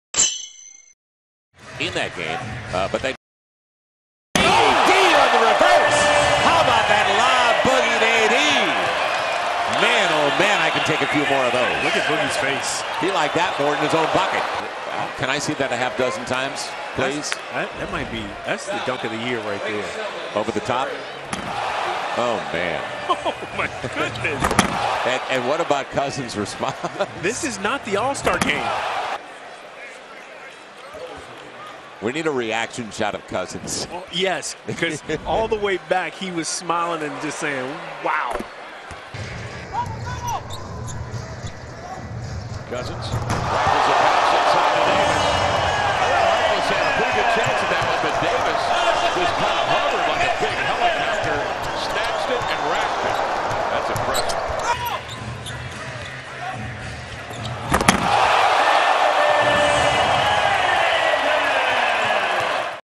Anthony Davis Aley-Oop Dunk vs Portland Trail Blazers March 2017
NBA League Season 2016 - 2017

NBA Highlights
Video Clip
Dunk
Plays
Recap
Random Player Highlights
Full Game Highlights
NBA HD 720p